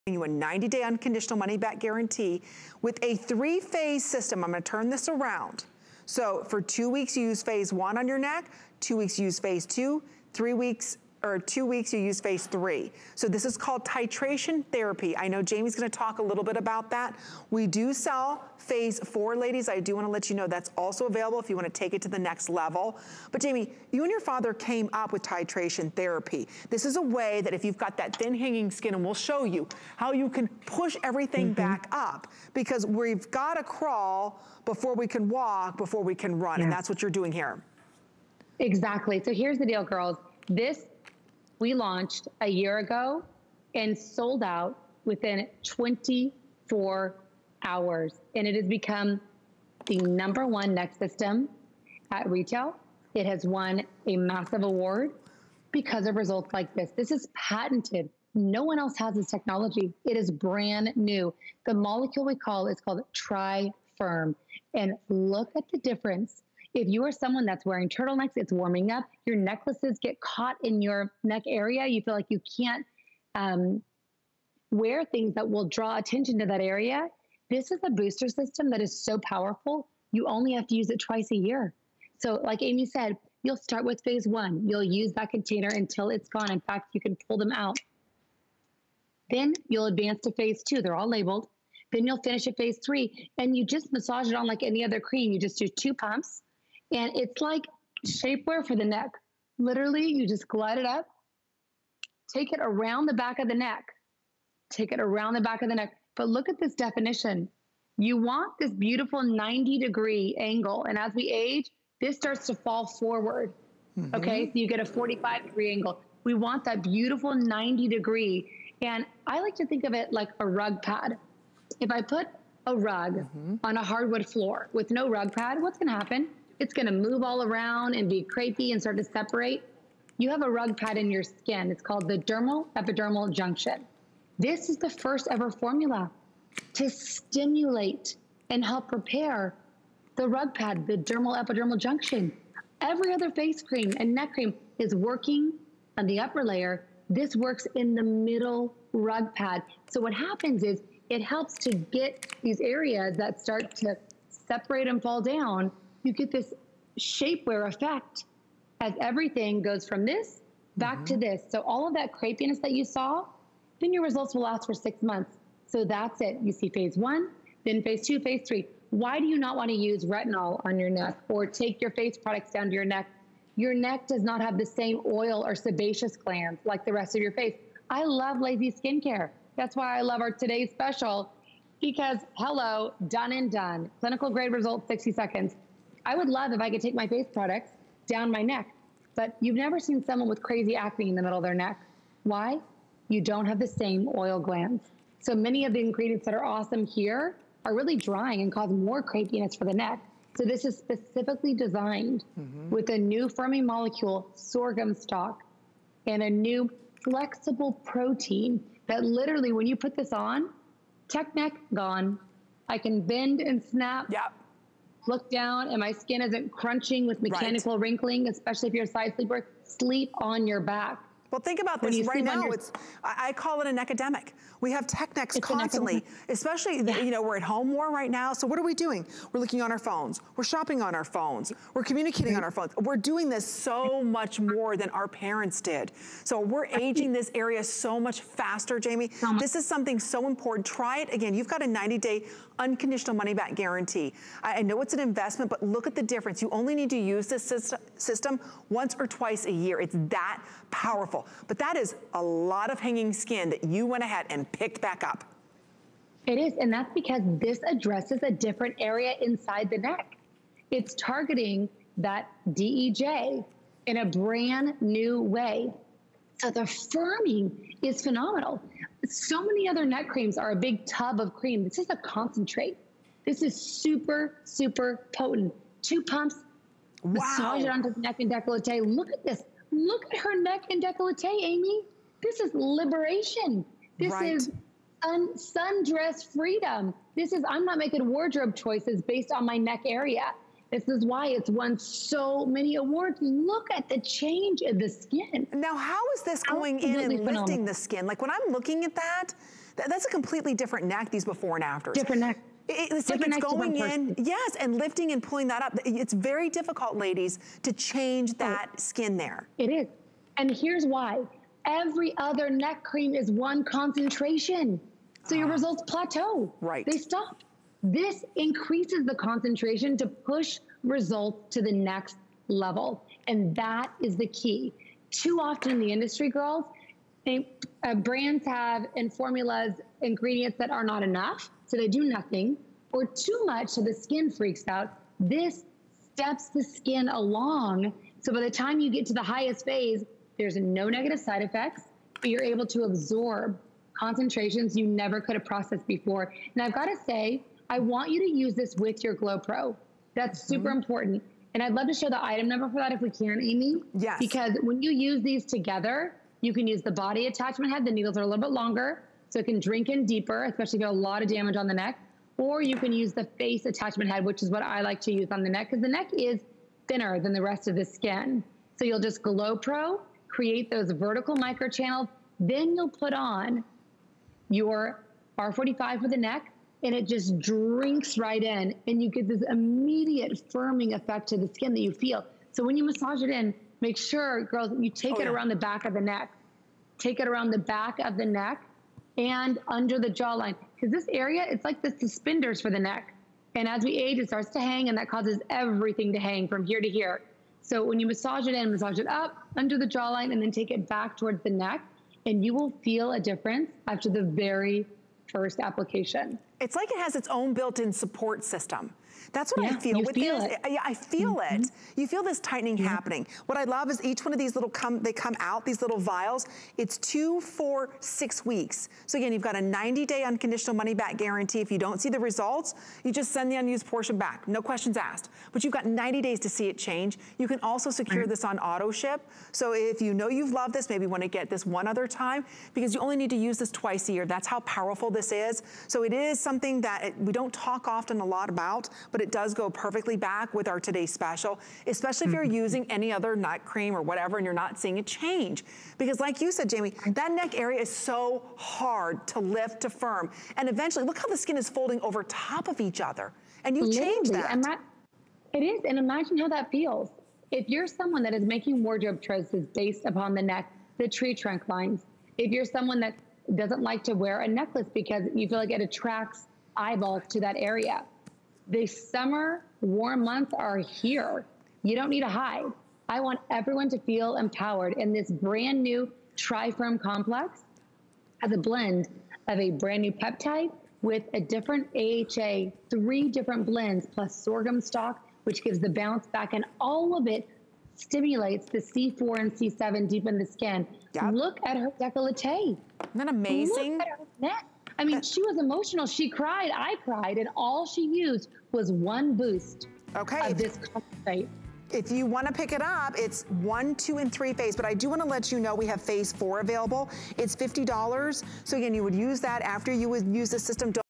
BeautyBio R45 The Lift 3Phase Neck Beauty System
R45 The Lift is a proprietary 3phase beauty treatment that helps give skin on the neck and decollete a younger, firmer and temporarily uplifted look with a reduced appearance of crepiness. Comes with a sample of The Quench, a moisturizing cream that helps deliver rich hydration and helps lock in and maintain skin's moisture levels.

    .17 fl. oz. R45 The Lift Phase 1
    .17 fl. oz. R45 The Lift Phase 2
    .17 fl. oz. R45 The Lift Phase 3
    .06 fl. oz. Sample packette of The Quench Restoring Quadralipid Cream

R45 3Phase System

    Proprietary 3phase beauty treatment with increasing concentration of TriFirm complex
    Contains complexes that have been shown to help improve the look of skin on the delicate neck and decollete area, resulting in a visibly youngerlooking, firmer, temporarily tightened, temporarily uplifted skin with reduced appearance of crepiness and a reduced look of uneven skin tone
    Includes TriFirm Complex, sorghum stalk, polylift 3D protein, spilanthes acmella, shea butter, cocoa butter, Vitamins A, E and F, and other key ingredients
    Helps provide hydration to skin for a smoother, softer feel

The Quench Restoring Quadralipid Cream

    Moisturenourishing, emollient cream that helps deliver rich hydration and helps lock in moisture and prevent transepidermal moisture loss
    Helps improve the appearance of radiance and texture
    Helps reduce the appearance of fine lines and wrinkles for youngerlooking skin
    Designed for dry skin types showing moderate to visible signs of aging

    The Lift 3Phase System  Recommended for evening use. Clean skin. Start with Phase 1. Dispense 1 pump of beauty treatment onto hand. Gently massage from middle of neck up to your jawline/chin. Dispense 1 more pump onto hand and gently massage from decollete to middle of the neck. Always apply product using a gentle upward motion. After completing Phase 1, advance to Phase 2 and then Phase 3. Use each phase once a day until each vial is gone, then proceed to next phase. Each vial should last around 15 days with some variance due to the pressure applied when dispensing. Overall treatment should last about 45 days. Follow directions provided on howto guide for best results. Results are cumulative. Do not start with Phase 3.

    The Quench Restoring Quadralipid Cream  Apply one dollop of cream directly on skin and massage gently. Can be used morning and evening. Avoid direct eye contact. Created to work both independently or layered after serums.